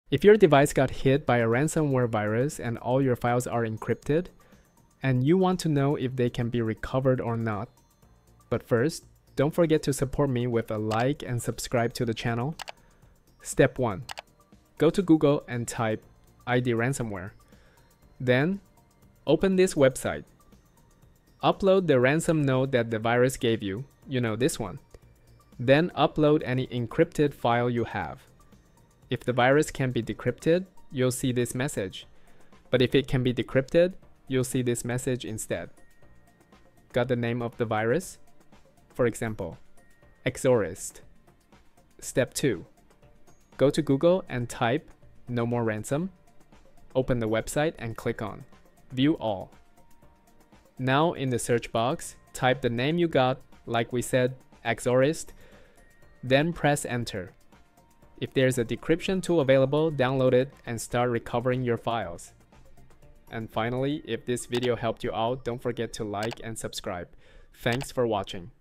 How To Decrypt Ransomware And Recover Your Files Ransomware Windows"
Got hit by ransomware? In this quick guide, learn how to identify the virus and recover your encrypted files for free. Works on most Windows systems!